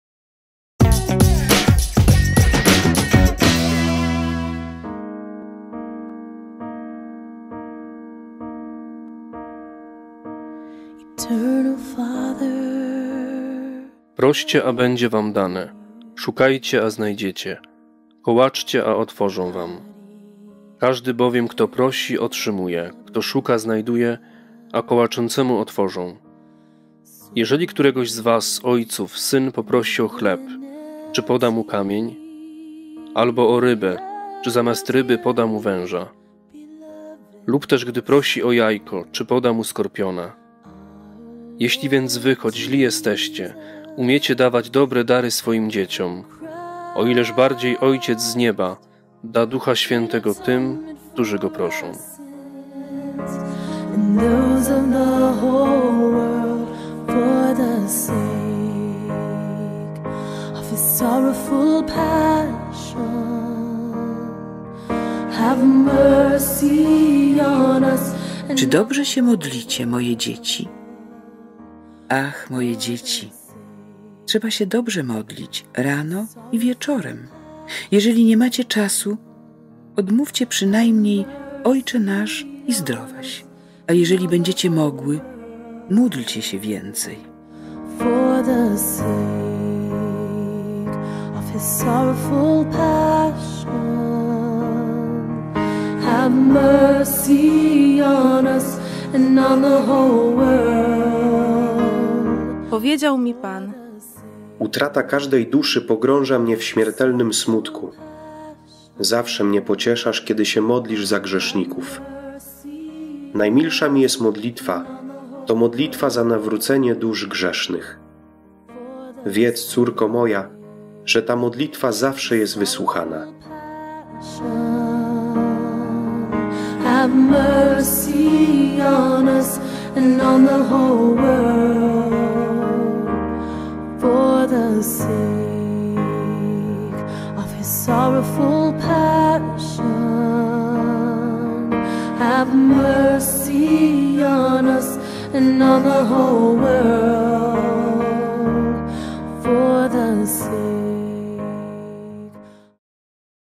MMS – Po co się modlić?
Po co człowiek ma się modlić? Czy modlitwa w ogóle coś daje? W La Salette Matka Boża pyta Maksymina i Melanię: „Czy dobrze się modlicie, moje dzieci? Ach, moje dzieci, trzeba się dobrze modlić, rano i wieczorem. Jeśli nie macie czasu, odmówcie przynajmniej 'Ojcze nasz' i 'Zdrowaś', a jeśli będziecie mogły, módlcie się więcej”. Modlitwa na pewno jest miłosiernym uczynkiem, szczególnie modlitwa o nawrócenie grzeszników.